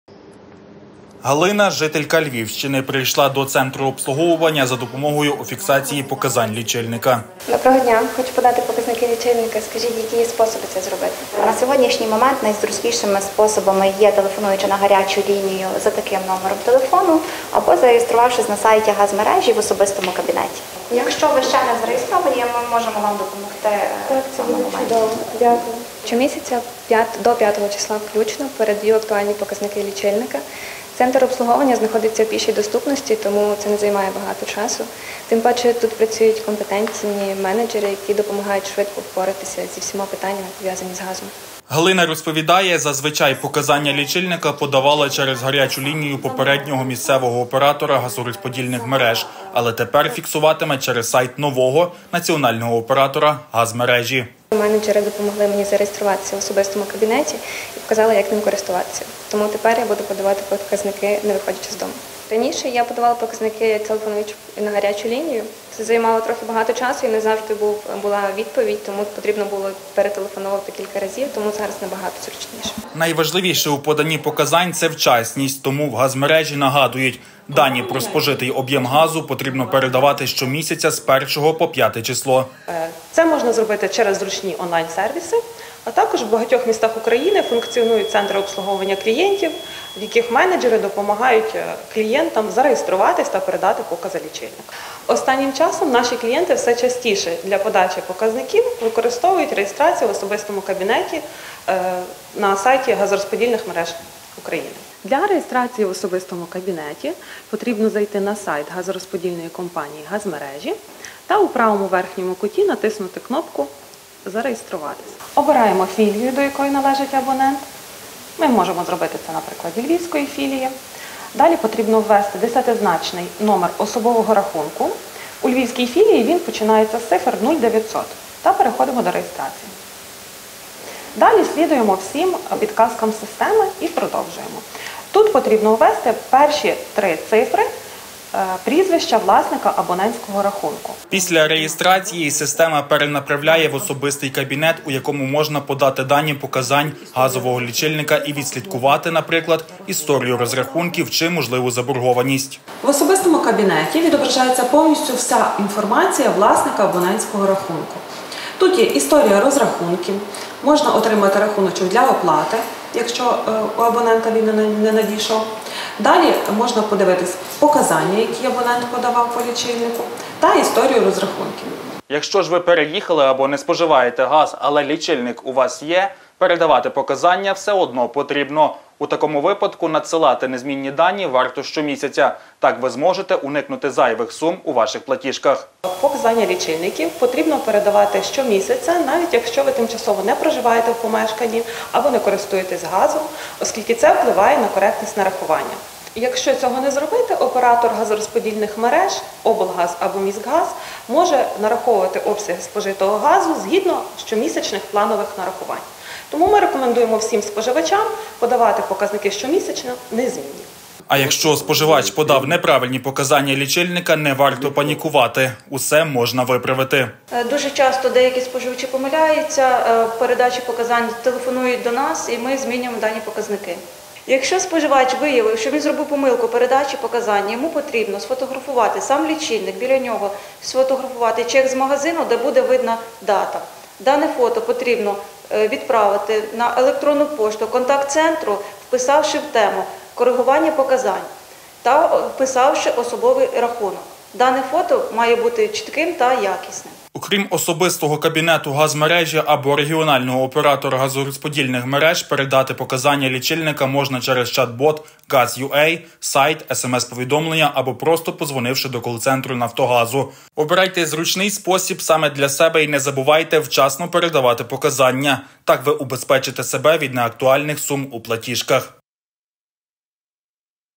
Все для зручності! Що вигадали газовики, щоб допомогти клієнтам
Сайт 104.ua більше не діє для передачі показників лічильника, тож які є альтернативи і який спосіб може підійти для кожного - дивіться в сюжеті.